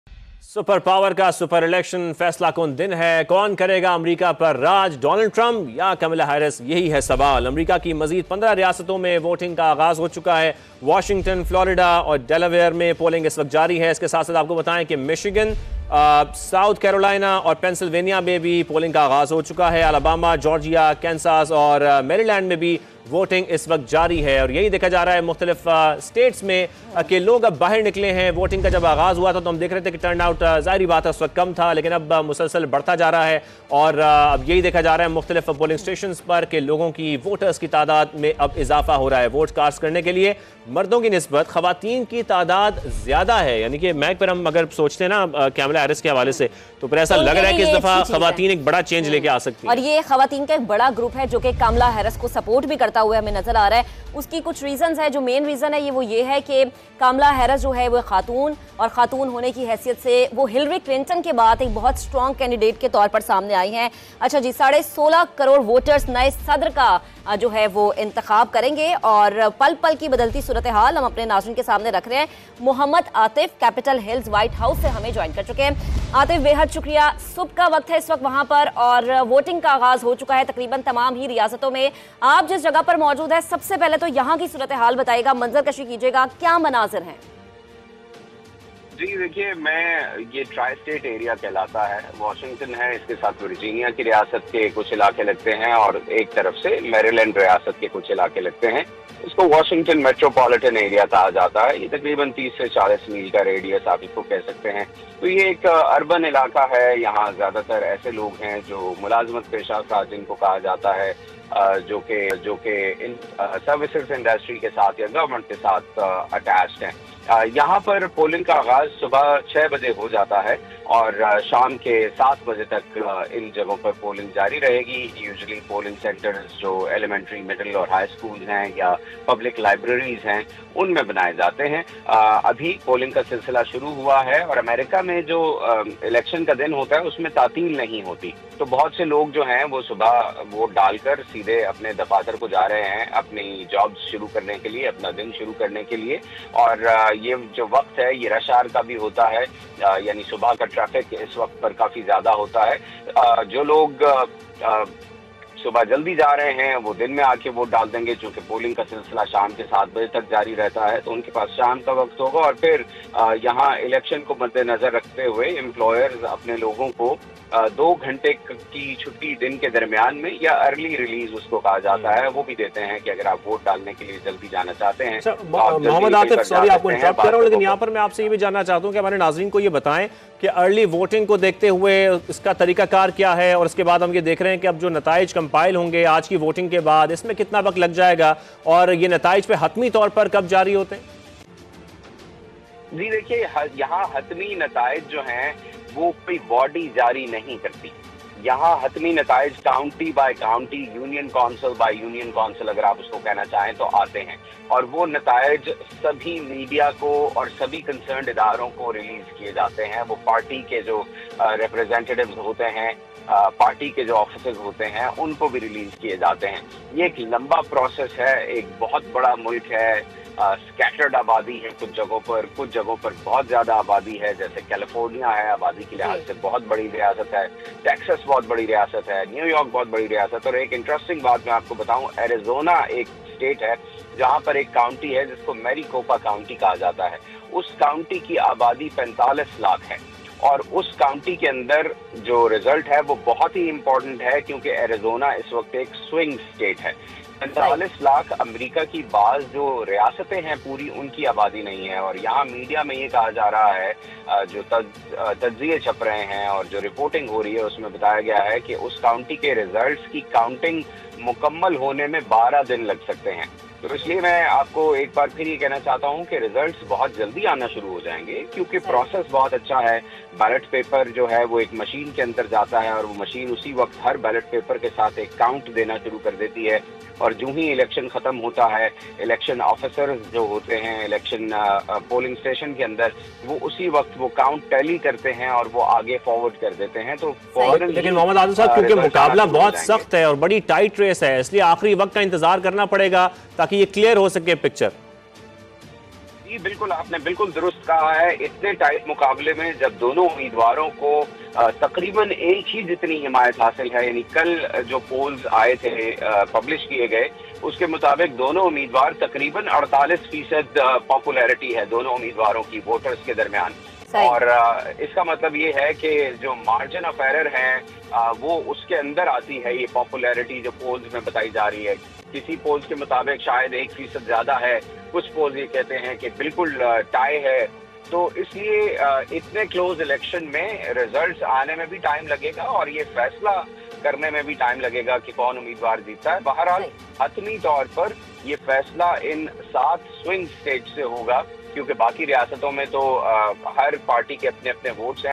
US election 2024 : Kamala Haris vs Donald Trump | Polls Underway | Game Change State | Samaa TV
Harris and Trump Battle to the Wire in Swing States, Times/Siena Polls Find

US election 2024 live: Latest polls tight as Trump, Harris hit swing states

US election: 2 days left – What polls say, what Harris and Trump are up to